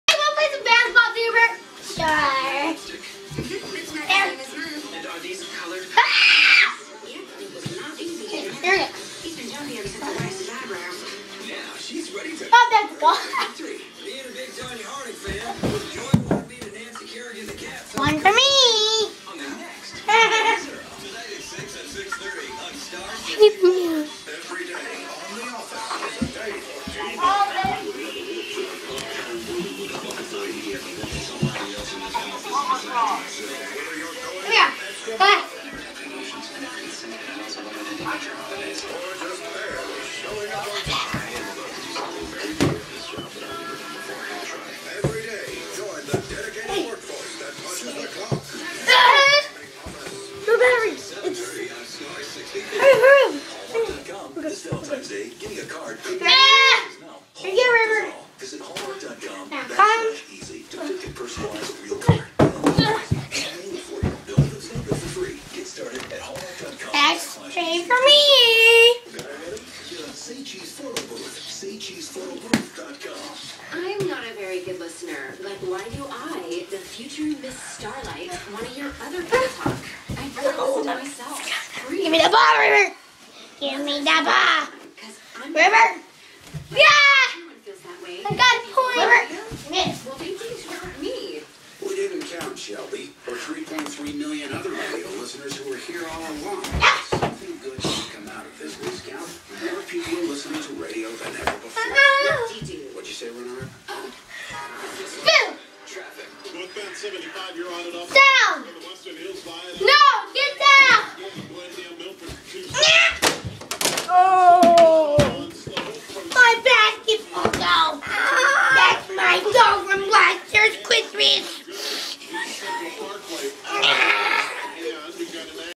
Rubert plays basketball
Rubert really shouldn't play basketball!!! He's the worst player ever! He sucks!